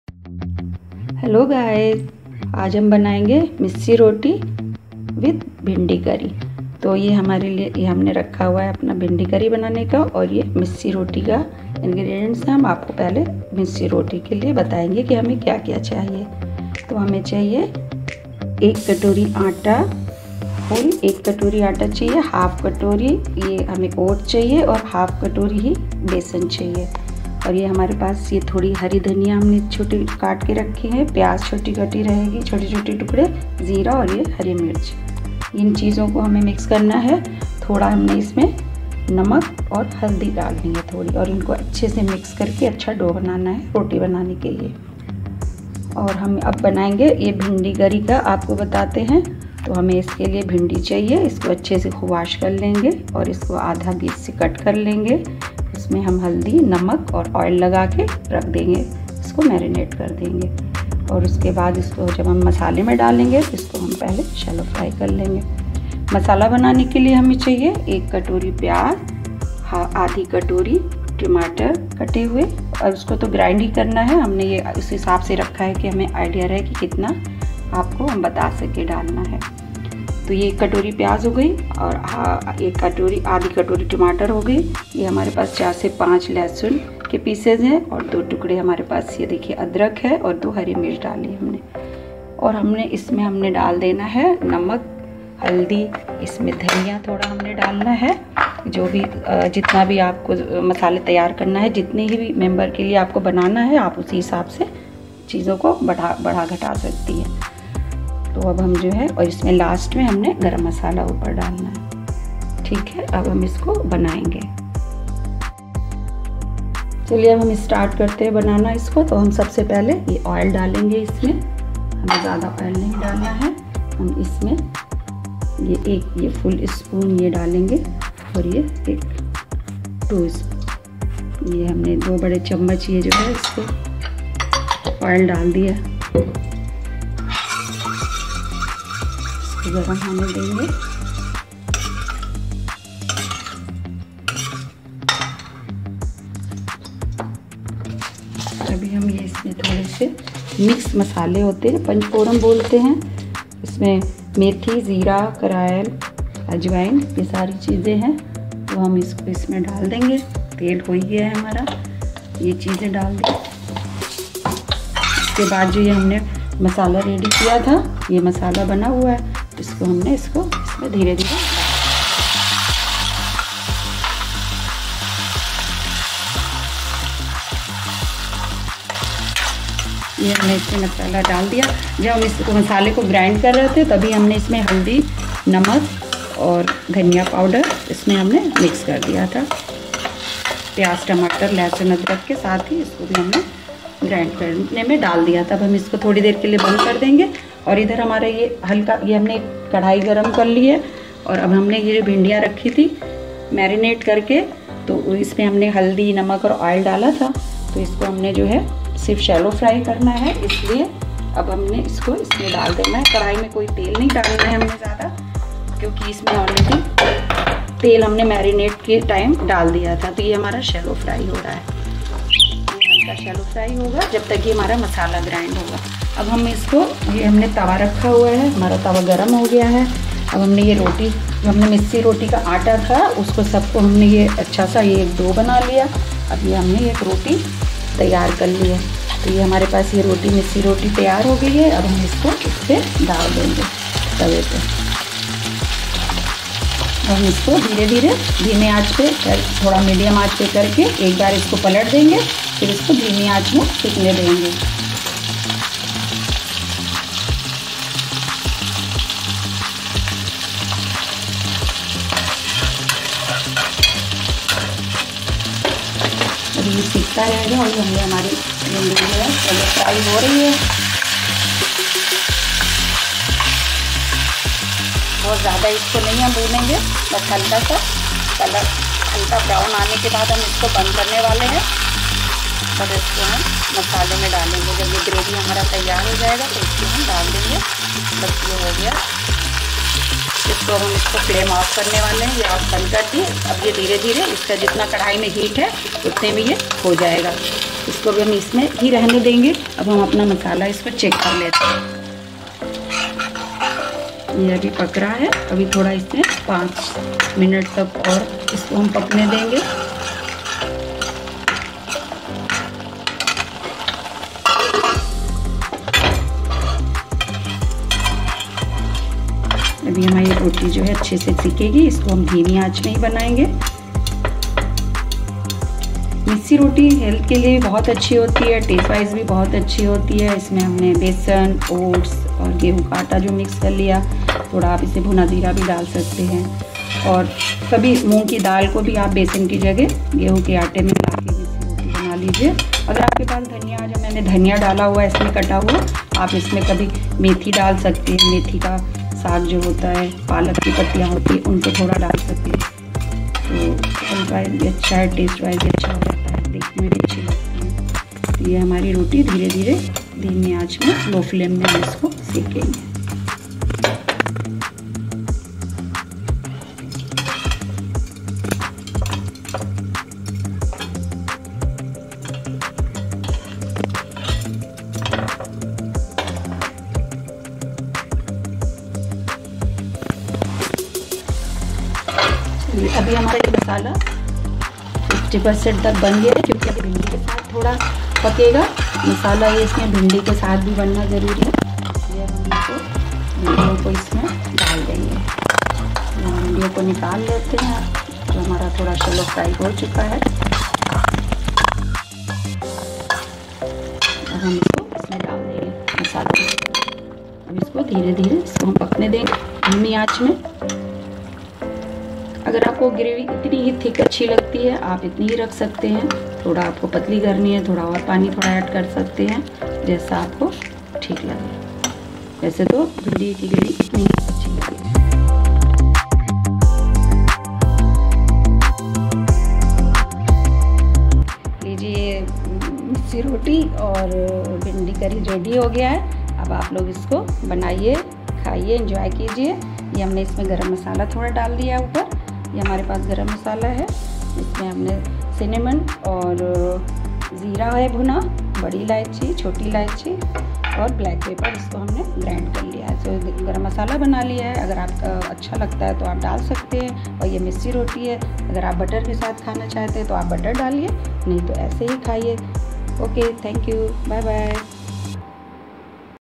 Masala Bhindi/Bhindi Curry ! मसालेदार भिंडी with मिस्सी रोटी।
Missi Roti with Bhindi Curry in Hindi !
Healthy Vegetarian Recipe !
भिंडी करी के साथ मिस्सी रोटी !
मसालेदार भिंडी with मिस्सी रोटी।
Today I am bringing to you one of my family favorites, Kadhai Bhindi and Missi Roti. This combination is not only exciting but also healthy. If some members of your family are not into Bhindi that much, please test their resolve on this dish, they don't stand a chance.